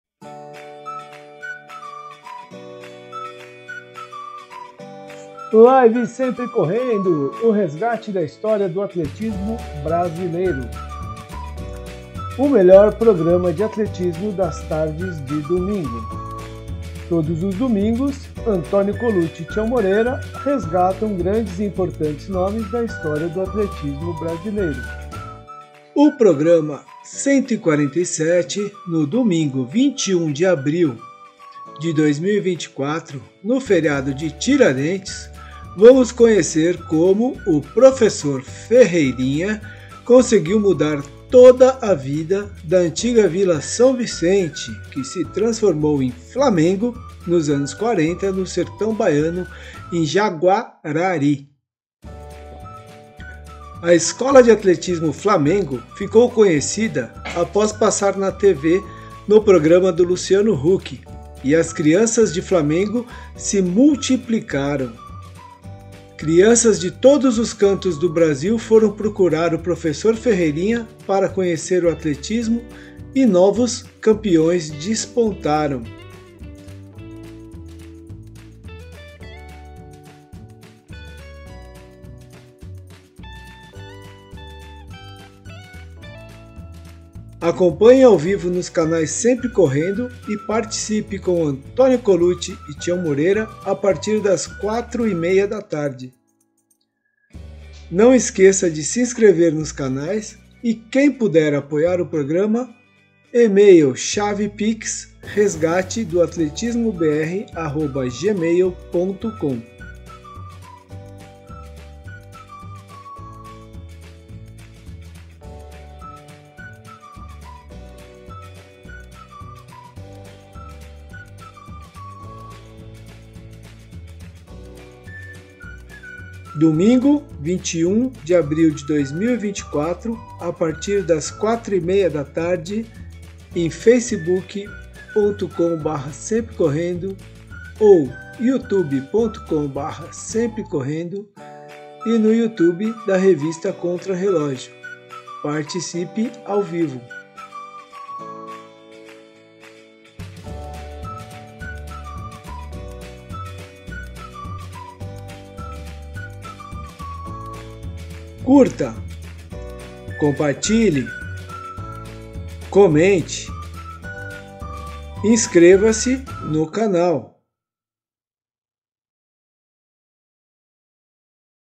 Prof. Ferreirinha - Antonio Ferreira Bonfim Filho - Escola de Atletismo Flamengo - Jaguarari/BA
(O trecho da apresentação foi bloqueado)

Com essa apresentação GLOBAL(CORTADA PELO YOU TUBE) ficou fácil saber quem é o nosso convidado para o Resgate da História do atletismo Brasileiro.

O programa 147 será no domingo, 21 de Abril de 2024, o feriado do Tiradentes e vamos conhecer como o professor Ferreirinha conseguiu mudar toda a vida da antiga Vila São Vicente que se transformou em Flamengo nos anos 40, no sertão baiano, em Jaguarari.

A Escola de Atletismo Flamengo ficou conhecida após passar na TV, no programa do Luciano Huck e as crianças de Flamengo se multiplicaram, crianças de todos os cantos o Brasil foram procurar o professor Ferreirinha para conhecer o Atletismo e novos campeões despontaram.

Acompanhe AO VIVO nos canais SempreCorrendo e participe com Antonio Colucci e Tião Moreira a partir das 4 e meia da tarde.

Por Antonio Colucci
Intagram @anton10colucci

Facebook.com/SempreCorrendo
Instagram.com/portal_semprecorrendo
Deixe seu comentário - Se gostou, COMPARTILHE.